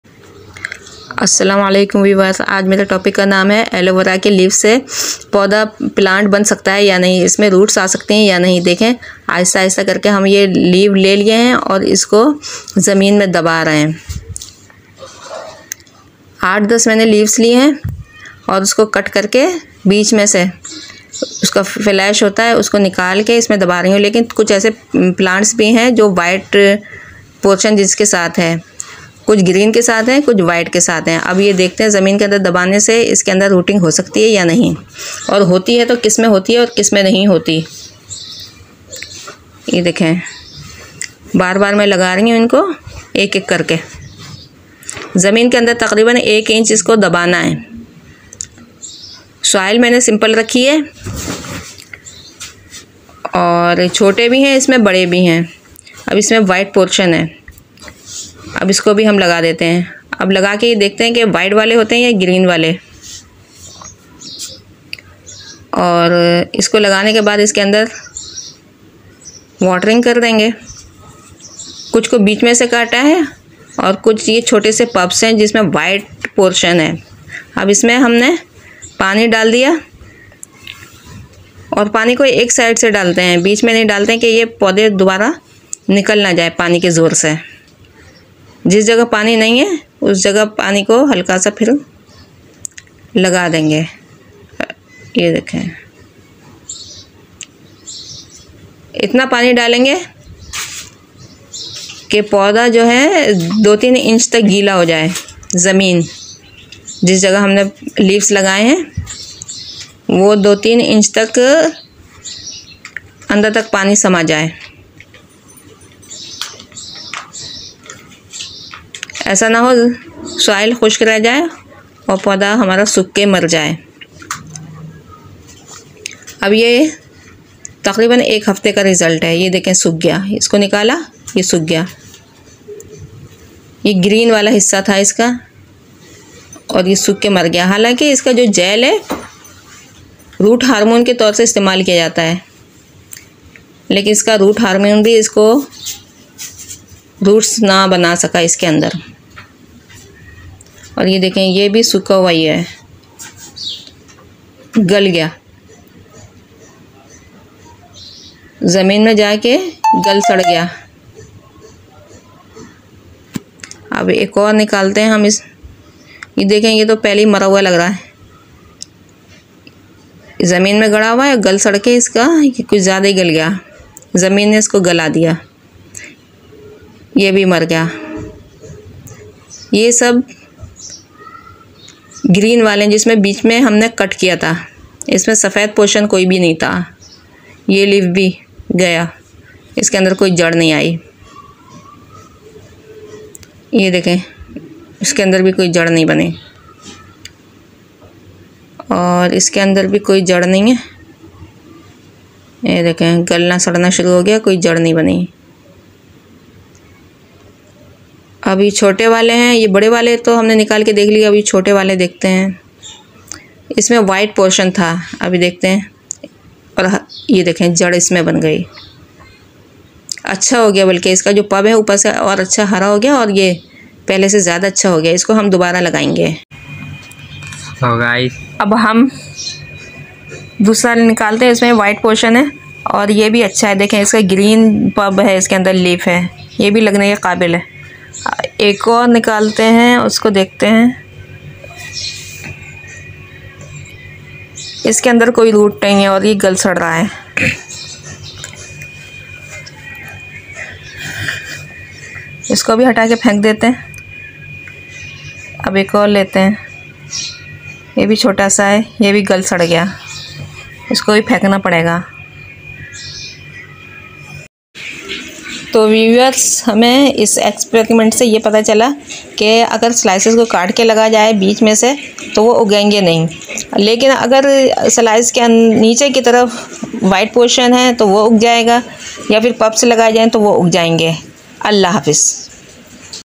propagation of aloe vera from single leaf//gardeningwithnayoutuber
hello assalamu alaikum. my topic name is propagation of aloe vera plant from single leaf, cut the leaves 🍃 from centre ⁰,sew in  a soil hole,watering. keep it in a shadow, cool  place.
After one week you⁰can see there is no rooting in the cut parts of the leaves, but that pup has  white  part it generated the roots.
propagation of aloe from single leaf.